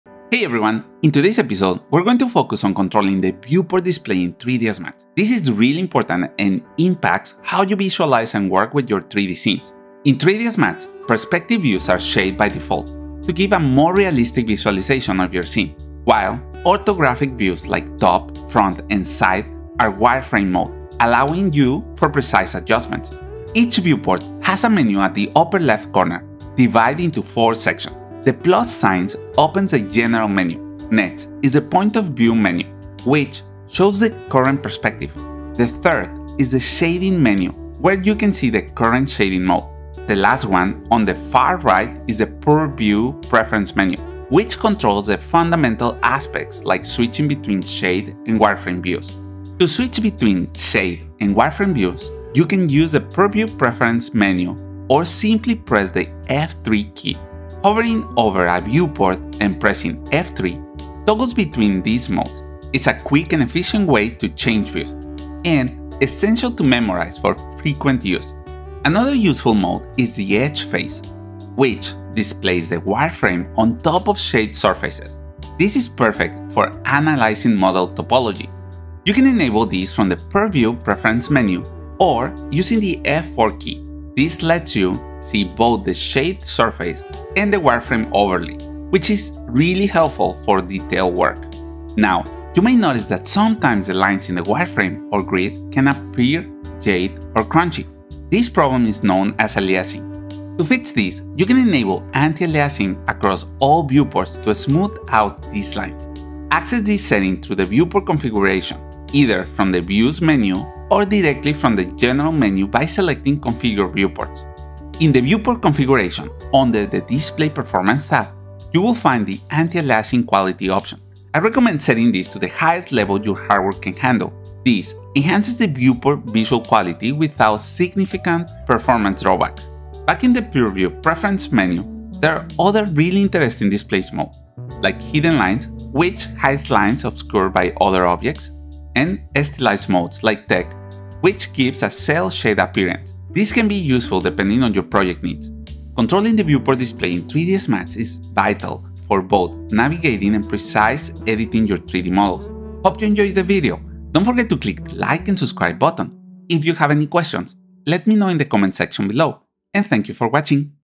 How to use the Viewport Shading Modes in 3DS Max
Learn how to effectively control the viewport display in 3DS Max. This skill is crucial for any 3D artist as it directly impacts how you visualize and interact with your 3D scenes.
We'll cover the essentials of shaded, wireframe, and edged faces modes, discuss how to toggle between them quickly, and delve into advanced settings like antialiasing to improve your viewport's visual quality.

🔥 What You'll Learn:
    How to switch between shaded and wireframe views using shortcuts and menus.
    The benefits of using edged faces mode for detailed modeling work.
    How to enable antialiasing to smooth out viewport lines and enhance display quality.

💡 Perfect for: 3D artists, designers, and anyone looking to enhance their 3DS Max skills by integrating assets and scenes with precision and ease.

CONTENT
00:00 Introduction to Viewport Display in 3DS Max
00:10 Display Modes: Shaded and Wireframe
00:23 Viewport Menu Overview
00:49 Switching Between Shaded and Wireframe Views
01:09 Edged Faces Mode
01:32 Fixing Aliasing: Viewport Configuration Settings
02:10 Other Display Modes
02:26 Conclusion